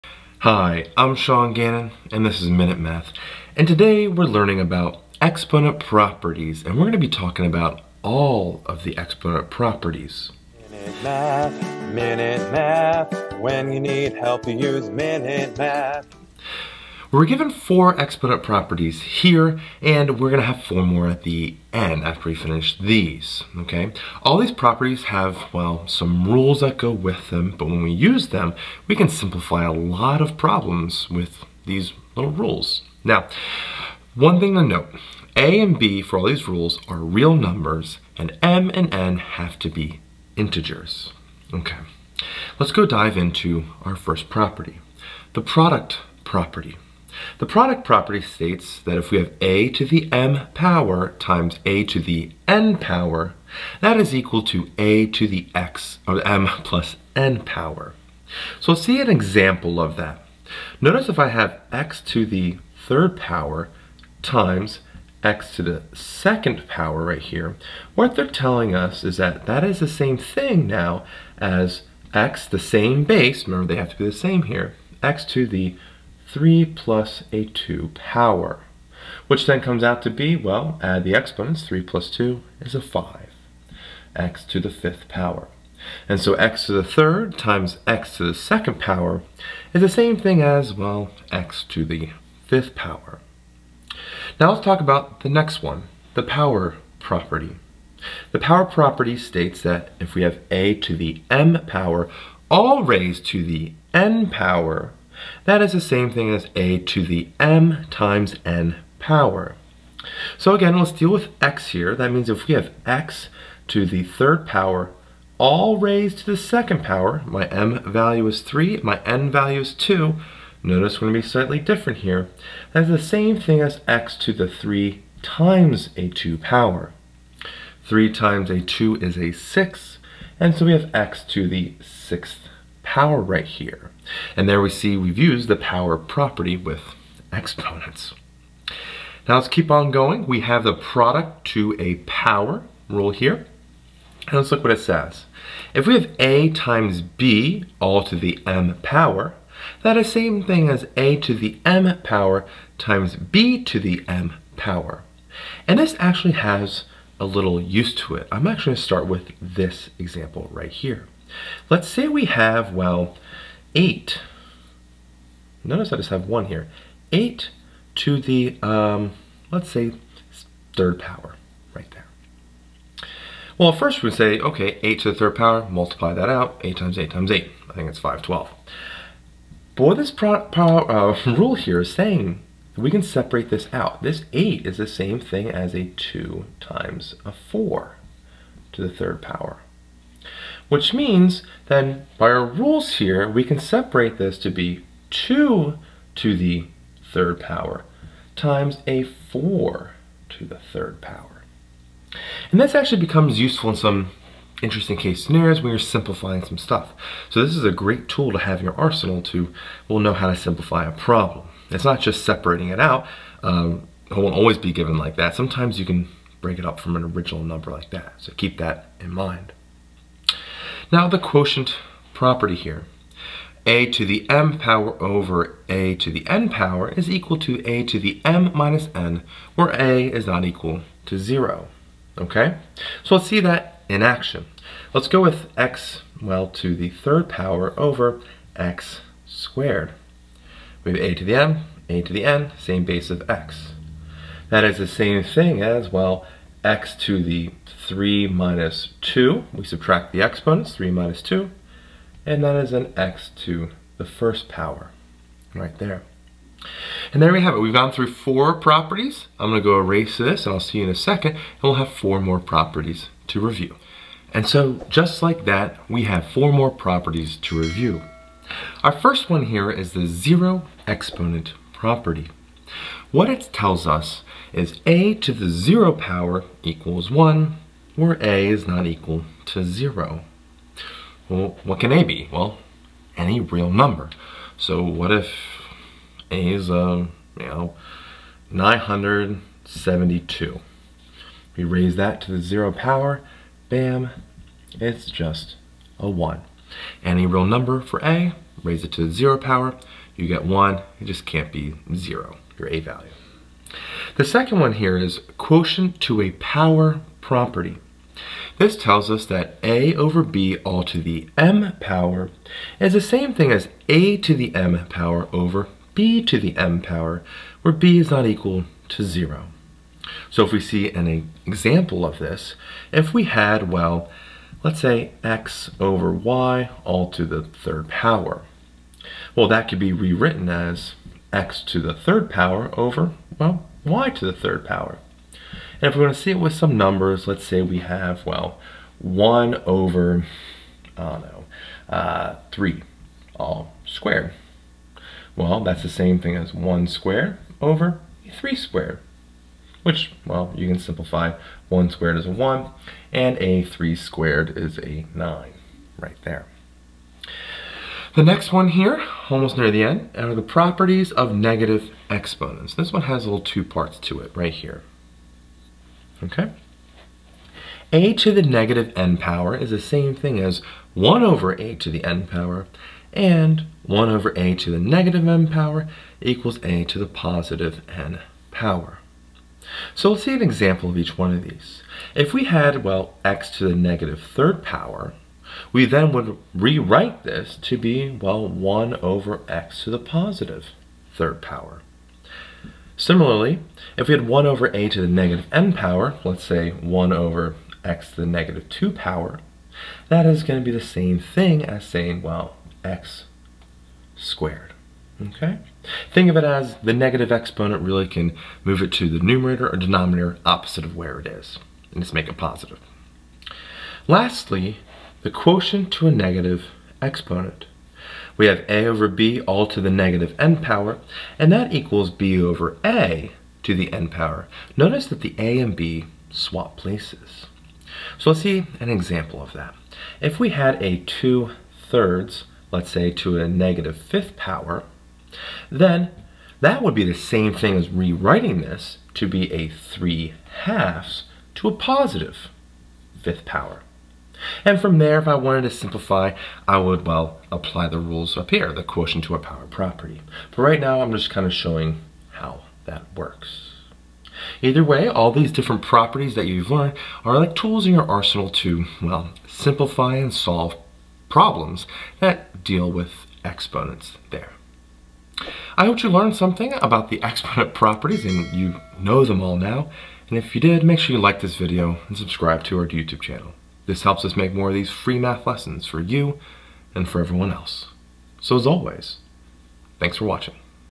Algebra Basics: Laws of Exponents | Minute Math
In this video we learn the different laws of exponents. There are 8 laws of exponents, sometimes called the properties of exponents. They are the Product Property of Exponents, the Power Property of Exponents, Product to a Power of Exponents, Quotient Property of Exponents, Zero Exponent Property of Exponents, Quotient to a Power Property of Exponents, Properties of Negative Exponents, and Quotient to a Negative Exponent.

Amazon Store Front: How we make our videos, Recommended Educational Resources, Etc.:

Marecek, L., & Mathis, A. H. (2020). Properties of Exponents and Scientific Notation. In Intermediate Algebra 2e. OpenStax.